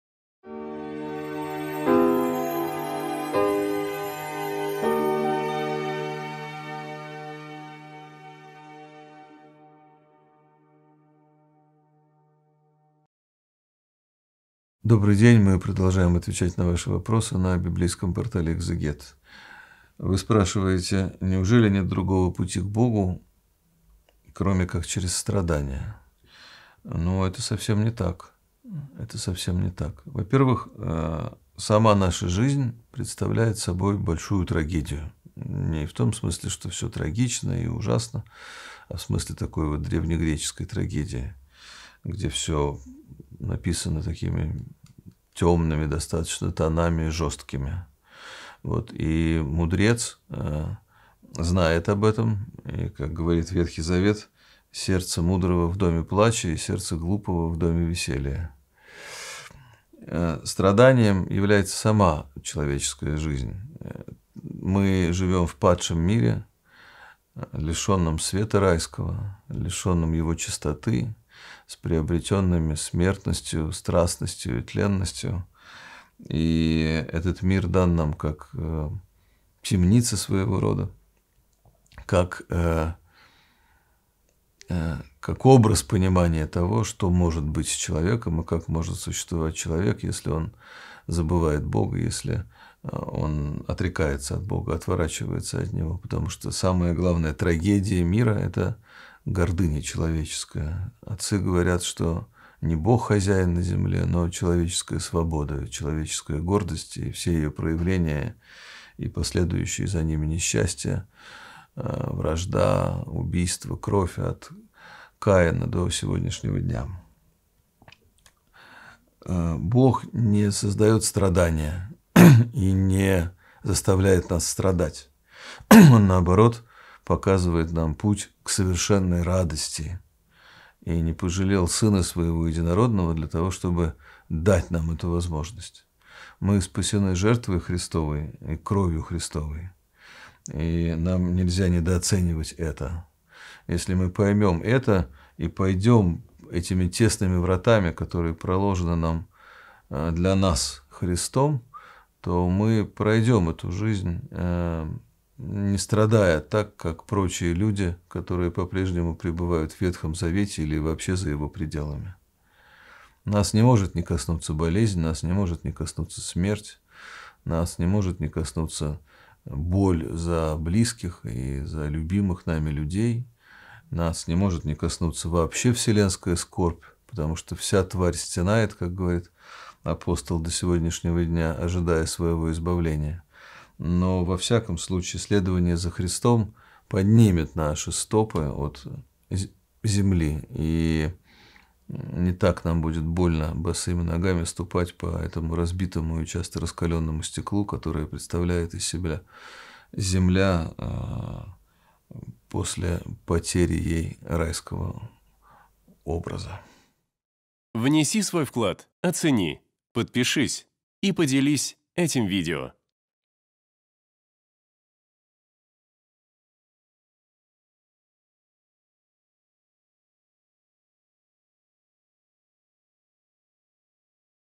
НЕУЖЕЛИ ПУТЬ К БОГУ ВОЗМОЖЕН ТОЛЬКО ЧЕРЕЗ СТРАДАНИЯ ? Протоиерей Дмитрий Рощин
Команда Экзегет.

В сегодняшнем видео на библейском портале Экзегет со священником Димитрием Рощиным размышляем над вопросом: "Неужели путь к Богу возможен только через страдания?"

🔸  Человеческая гордость. Страдания
🔸  Путь к совершенной радости
🔸   Спасение жертвой Христовой

📜 Читать книгу Екклесиаста главу 7, стих 4 (с толкованиями)